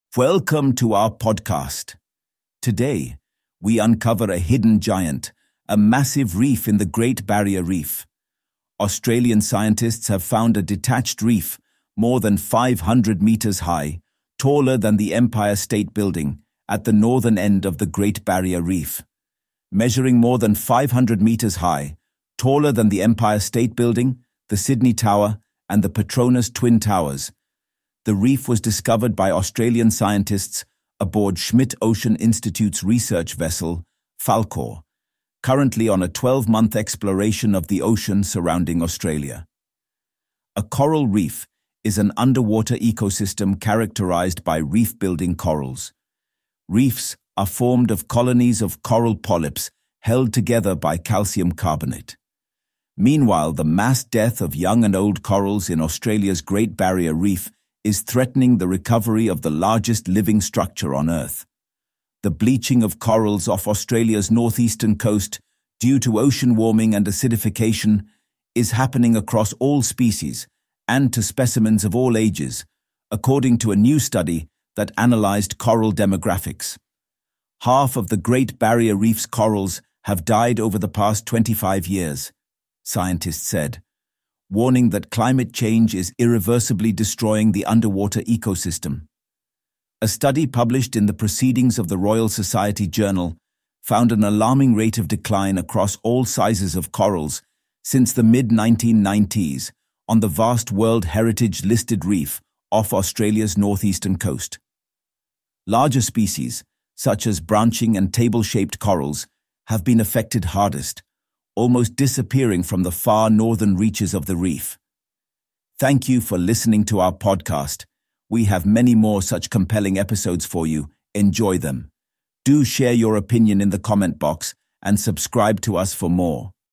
Giant Reef Taller Than Empire State Building Discovered! | WION Podcast
Australian scientists have discovered a massive 500-meter-tall detached reef at the northern end of the Great Barrier Reef—taller than the Empire State Building! Found during an ocean exploration mission, this reef adds to the wonders of our marine world. However, the Great Barrier Reef is facing a crisis, with half of its corals dying in the past 25 years due to climate change. Coral bleaching caused by warming oceans threatens this fragile ecosystem. Can discoveries like this inspire stronger conservation efforts? Dive in to learn more!